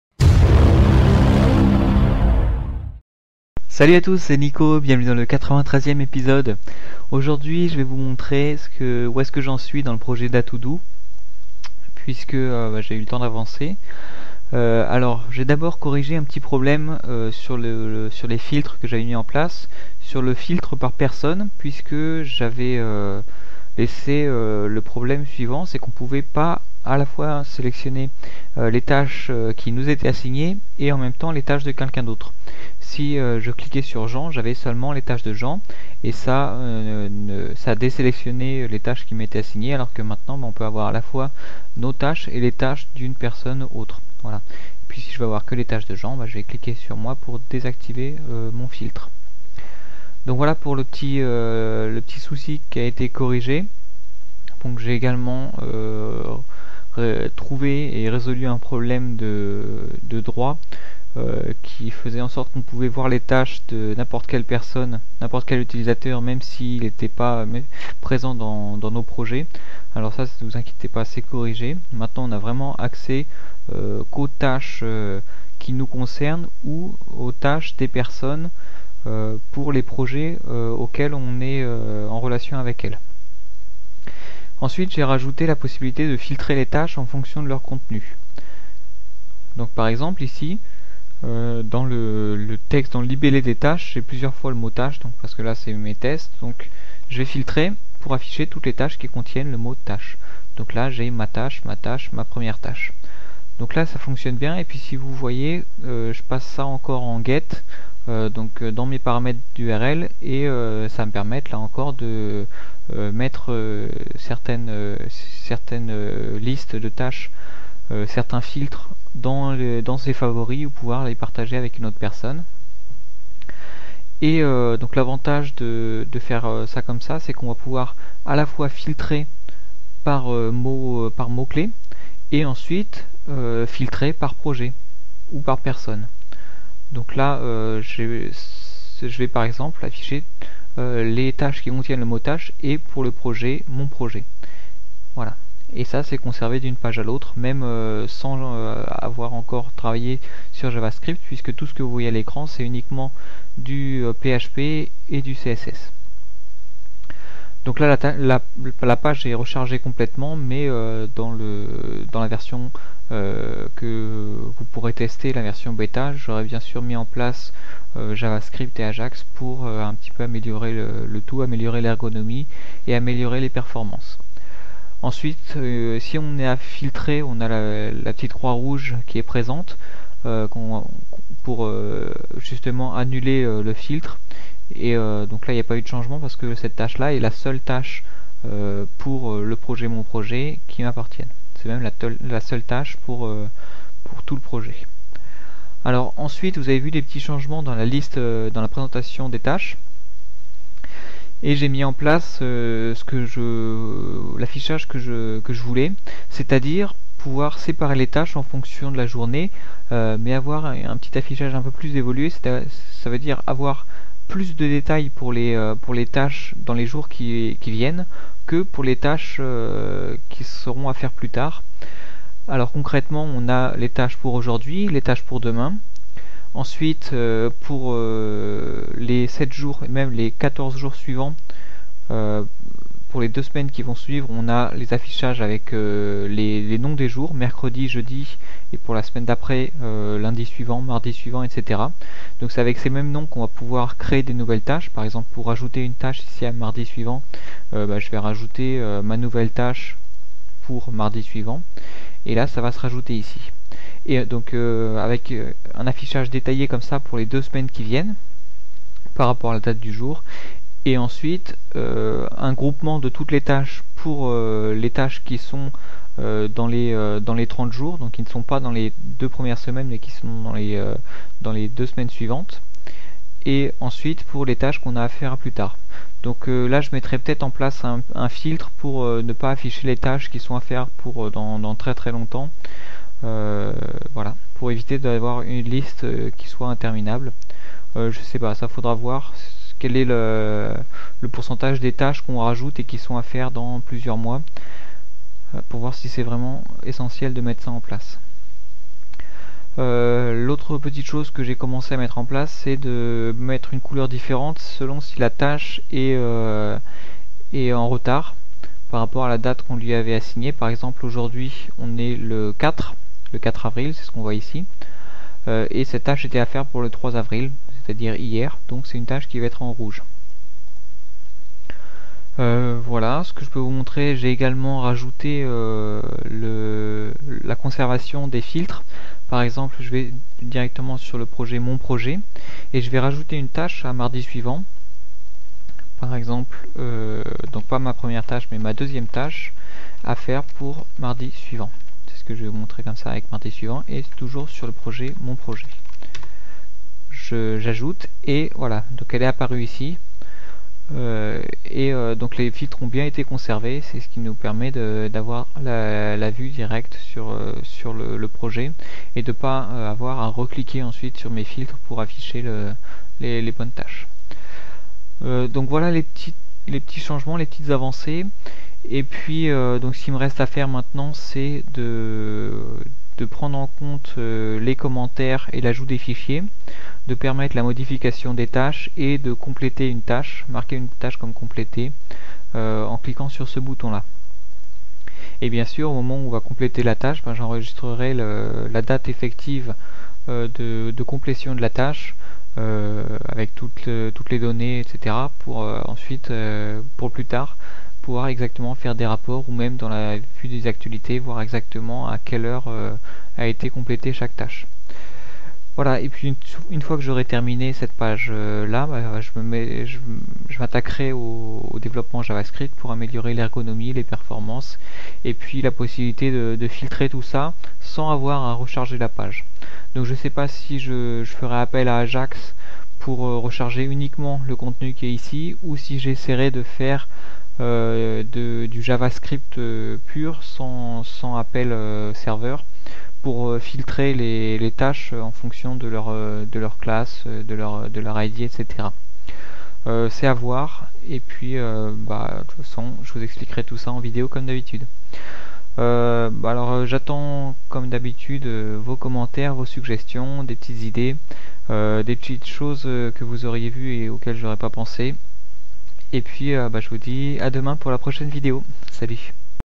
Episode n°93 - Affichage des tâches par date
Dans cette vidéo, je vous parle des différentes choses sur lesquelles j'ai travaillé pour le projet datodo, et notamment de l'affichage des tâches par date.